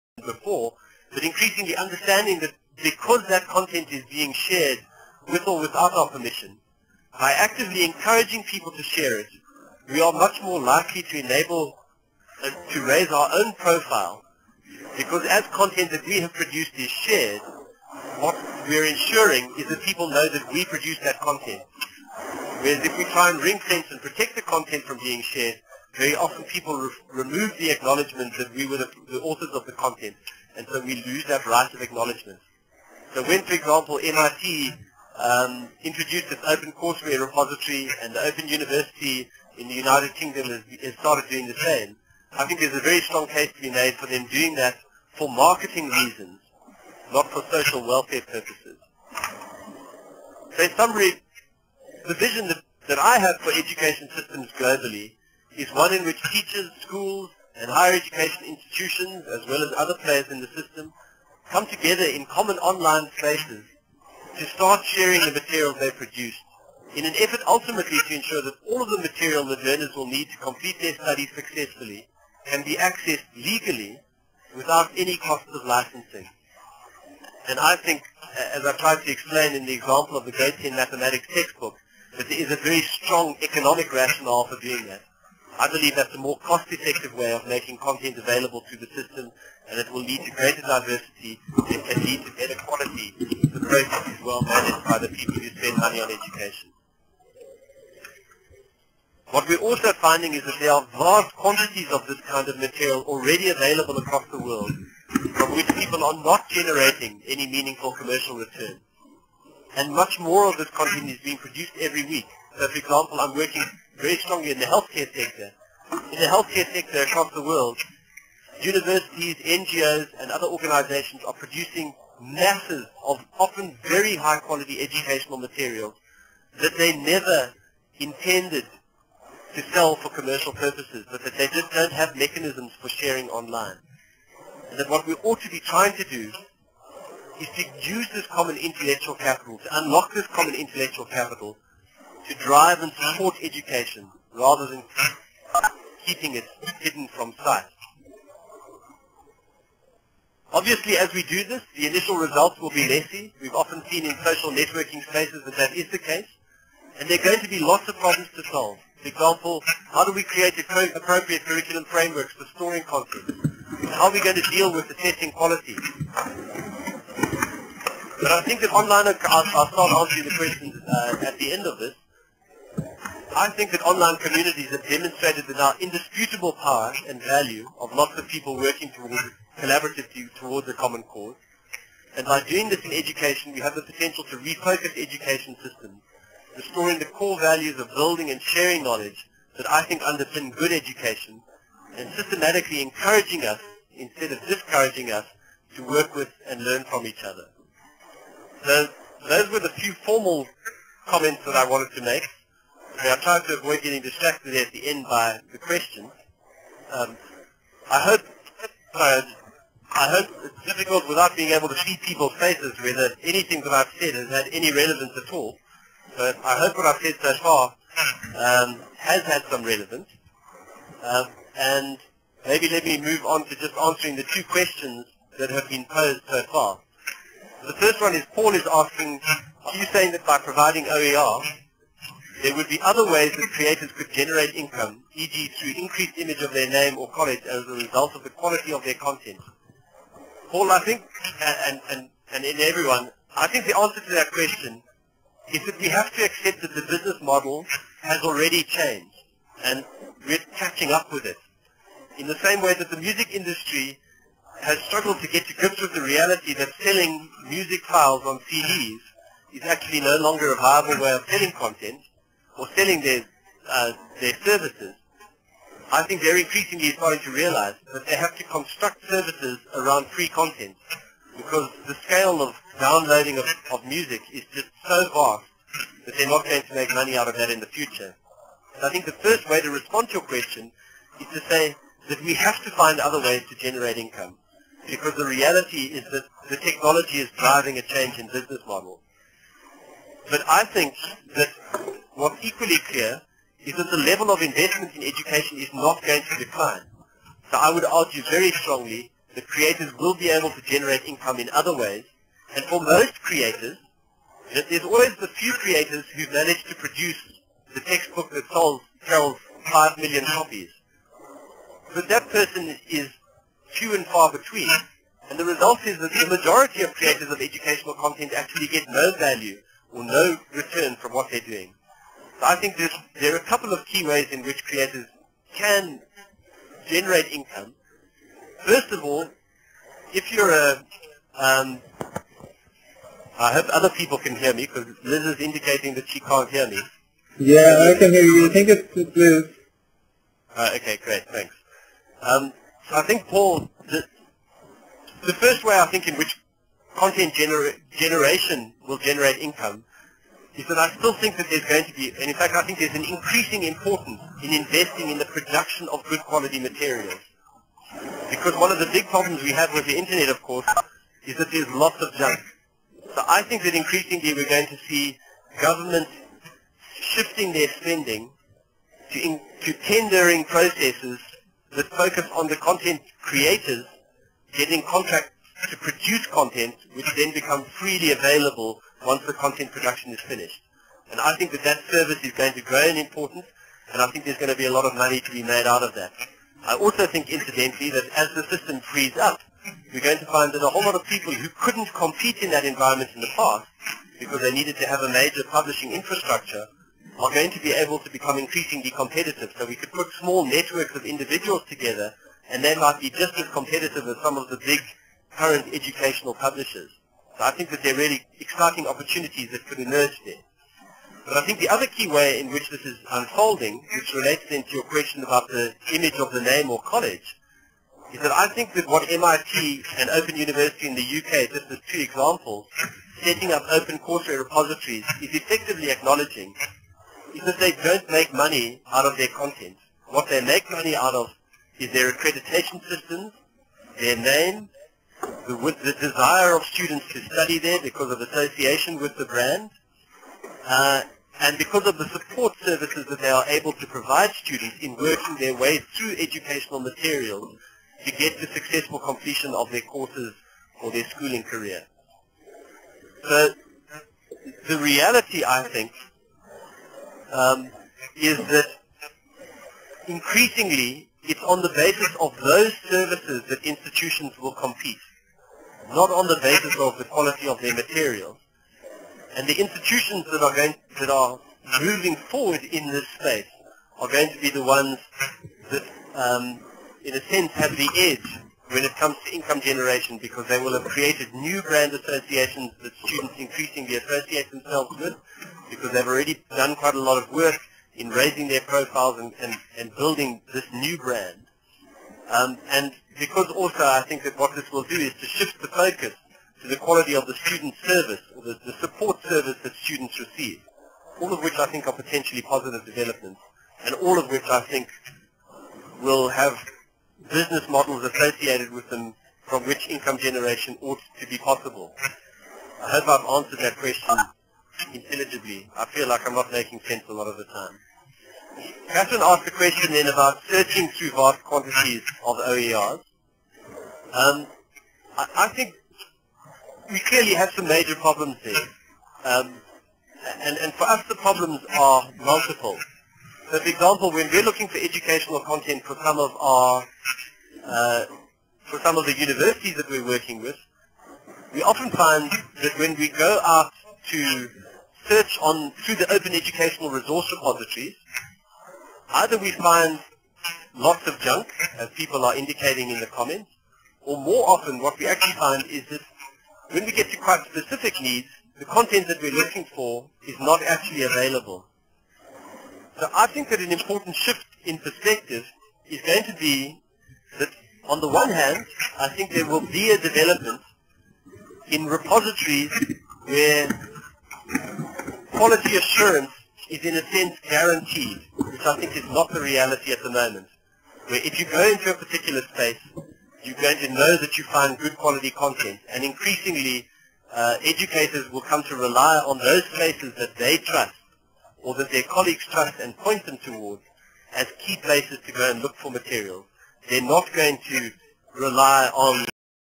Introducing OERs, Neil Butcher, OER Strategist, OER Africa (4/5)
Part Four of an online presentation introducing Open Education Resources and what they can do for education. The session is part of the 4C Initiative.